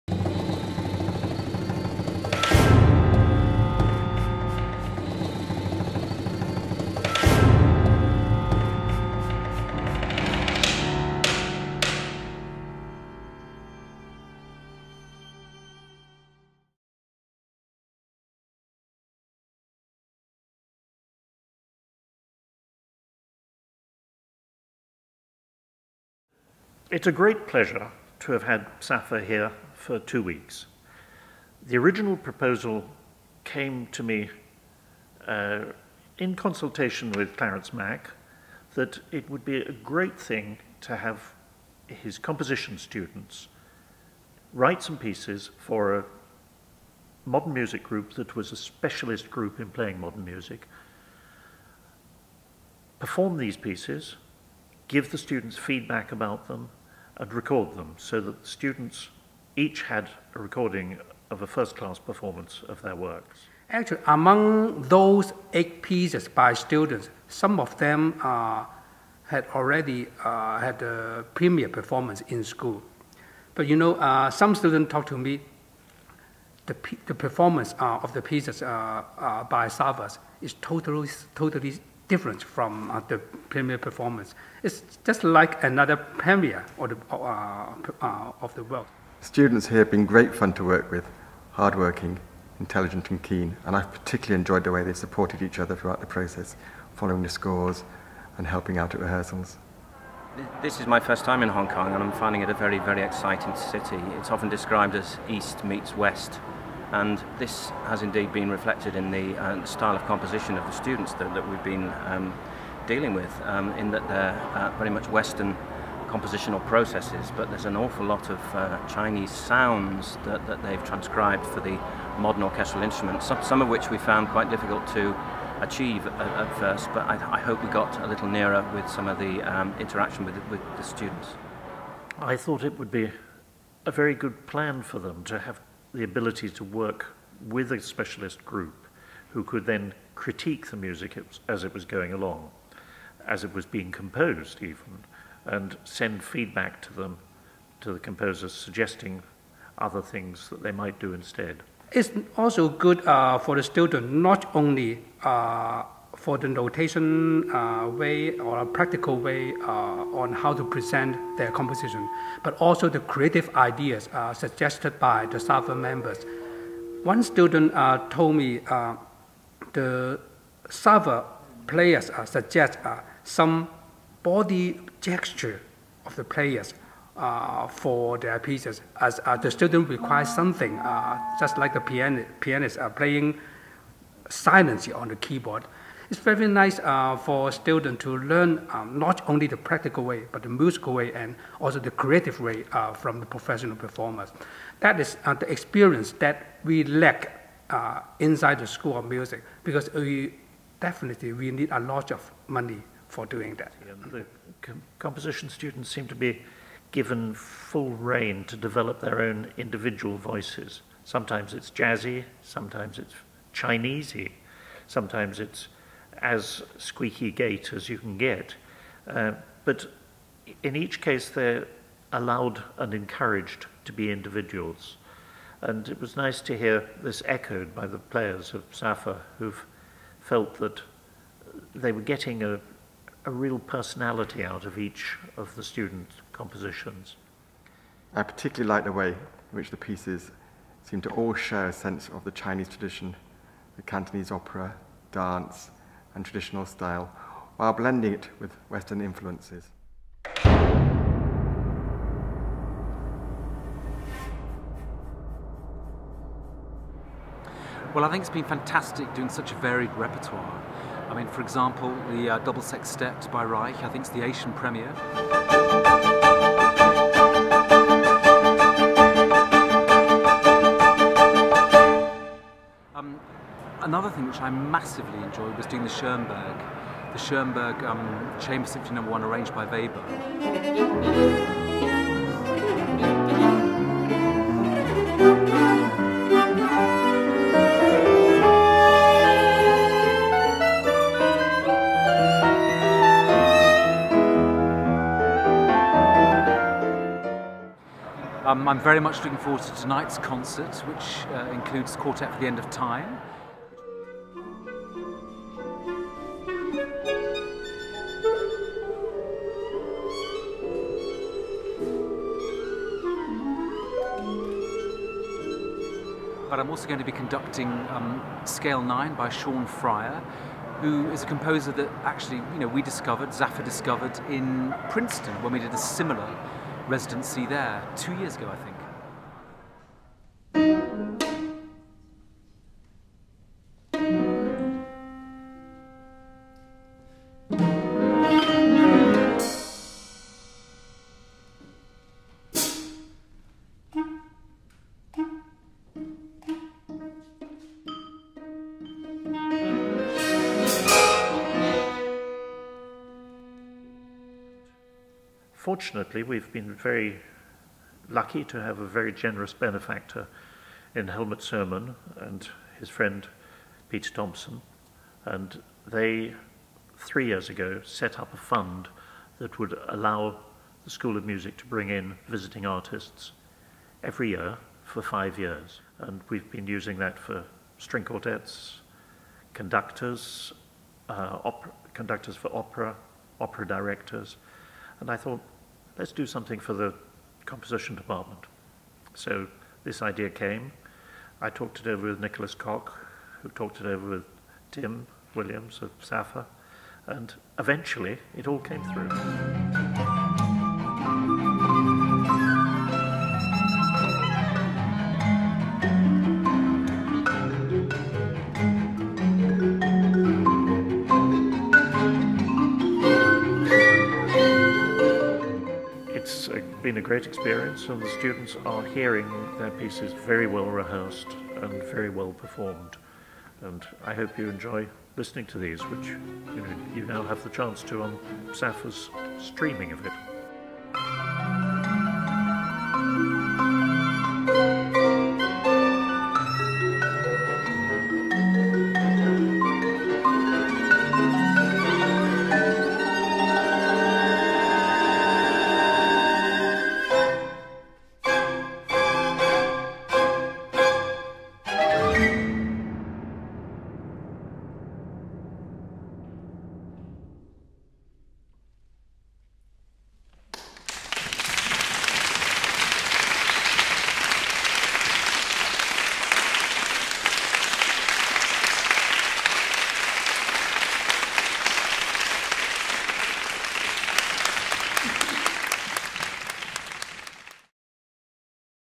Introduction to Psappha's Hong Kong Residency 2013
A short 8 minute introduction to Psappha's residency at The Hong Kong Academy for Performing Arts.